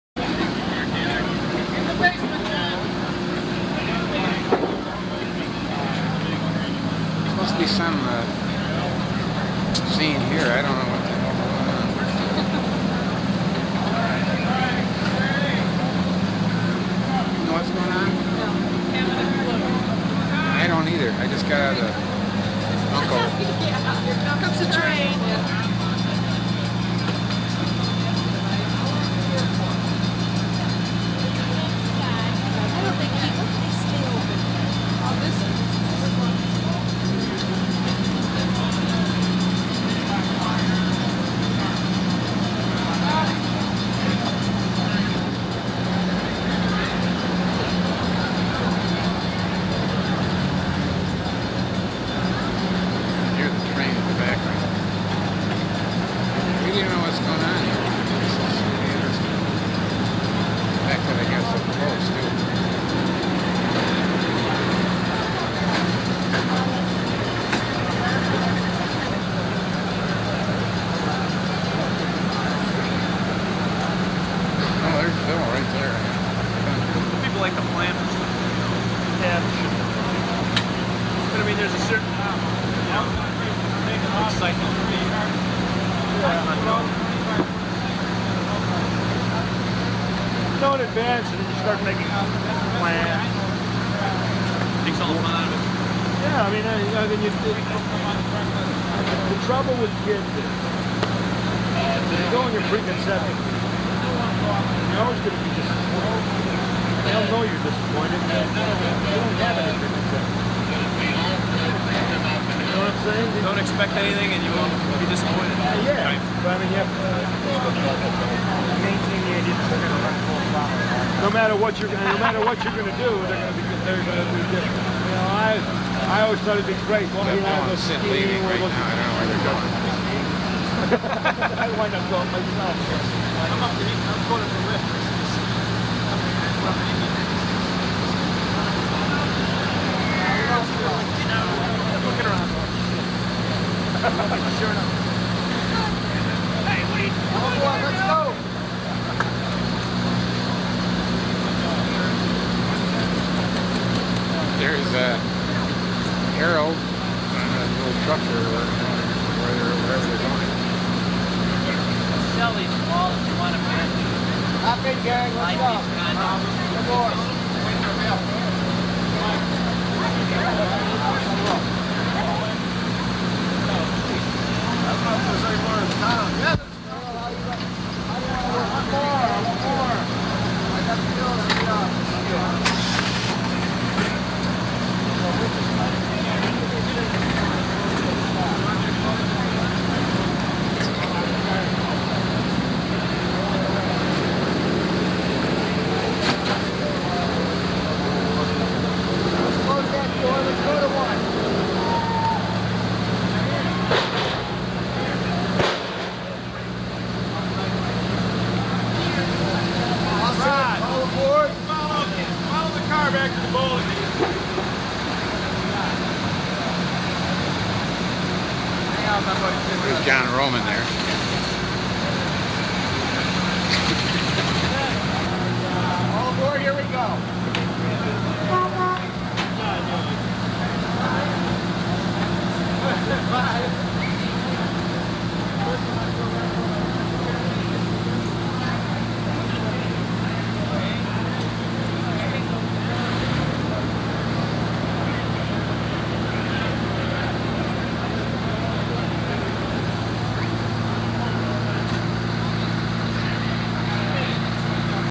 Groundhog Day Movie - Making of - Old Red Cadillac night scene
The filming of the movie Groundhog Day in Woodstock, IL in 1992.  In this scene we see the old red Cadillac that Bill Murray and his drunk bowling alley buddies are riding in being prepared for a film scene.  The camera truck is towing the Cadillac through the streets of Woodstock at night.   James Carvis was behind the camcorder.
Harold Ramis is seen directing the operation.